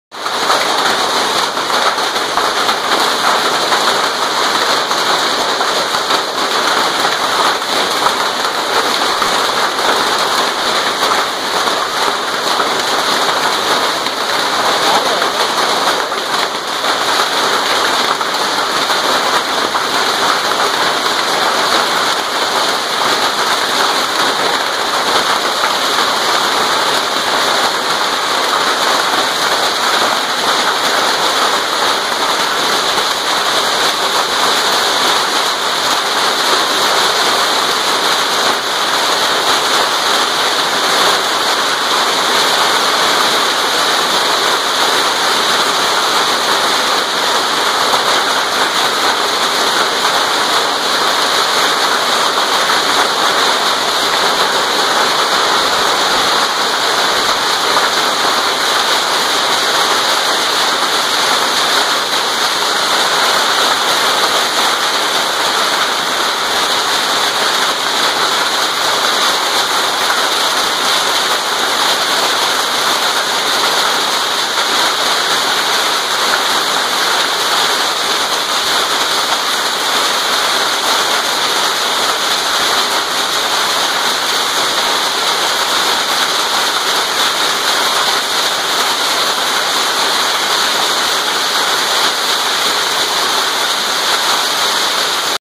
Espectacular caída de granizo en San Dionisio Ocotlán
Oaxaca. Así granizó en la comunidad de San Dionisio Ocotlán, algo que no se había visto